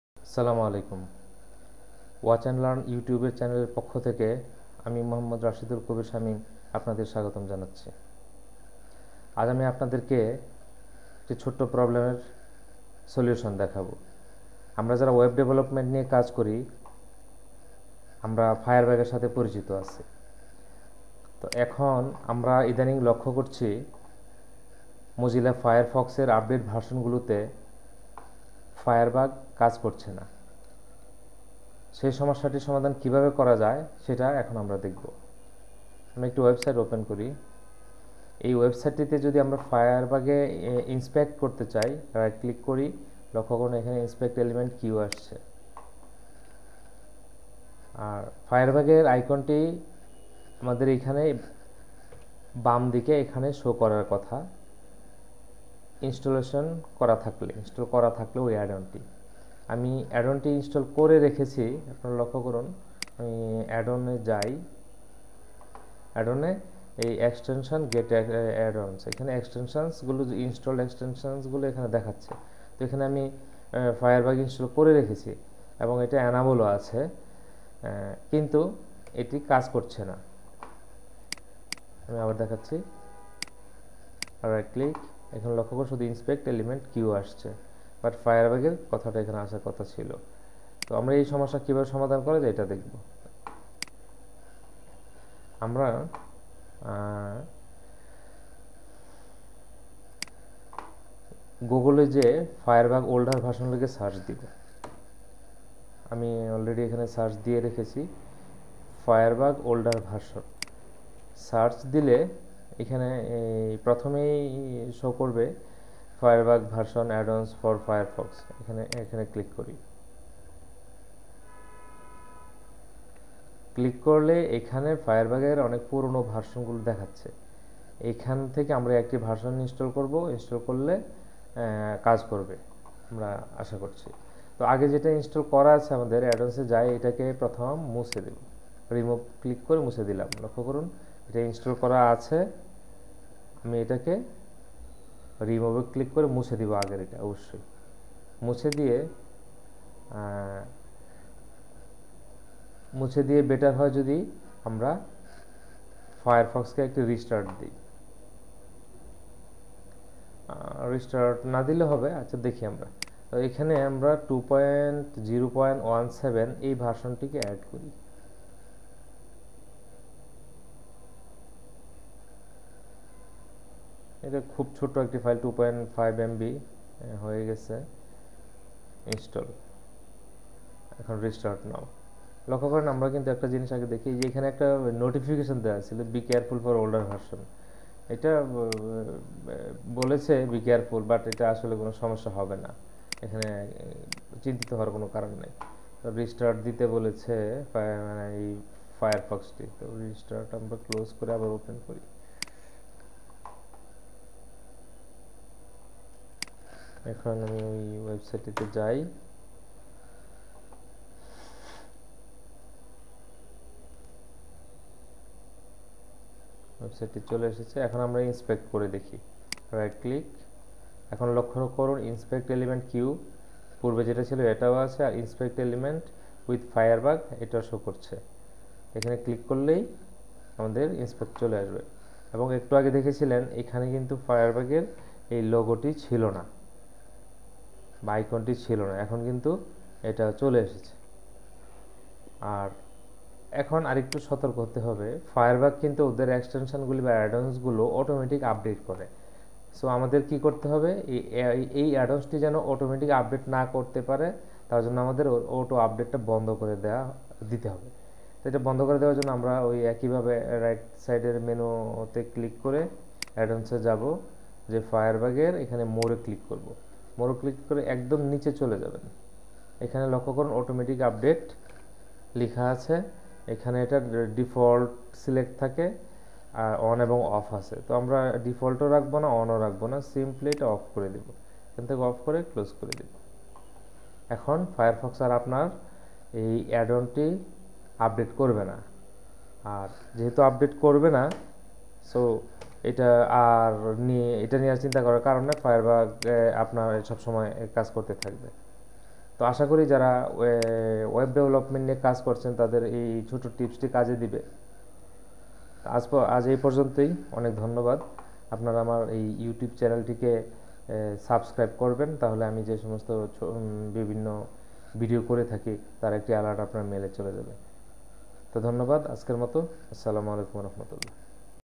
How to fix Firebug Problem in Mozilla Firefox Bangla - Watch & Learn
See the short video and solve the Firebug problem at easy way. How to install Firebug